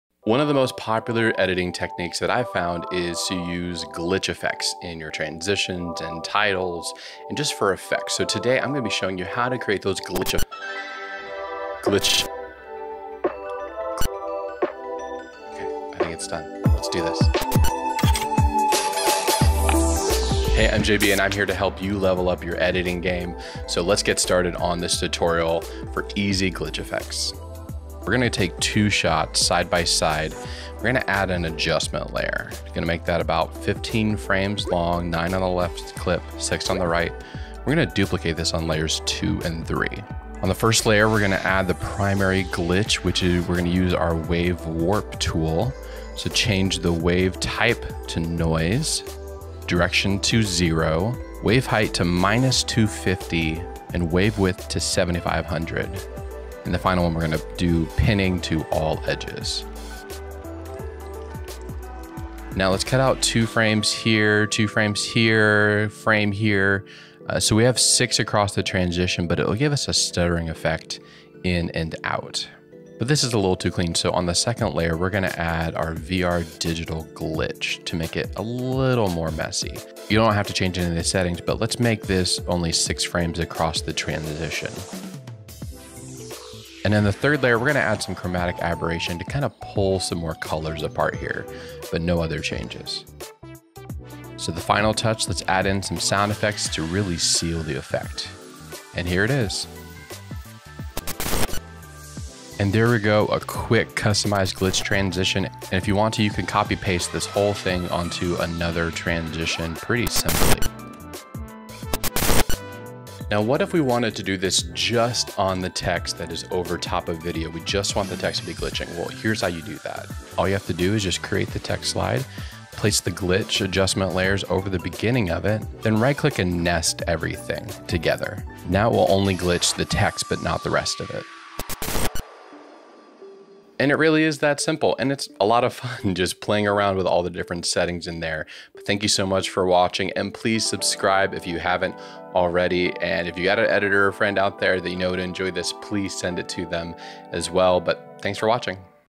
Glitch Effects in Premiere Pro you didn’t know about
How to make super simple Premiere Pro Glitch Effect without plugins or After Effects.

Shot On:
- GoPro Hero 8 Black
- Tascam Dr-60D mark ii
- Rode Podmic
- iPhone 8+

Edited with:
- Adobe Premiere
- 2018 Macbook Pro 16"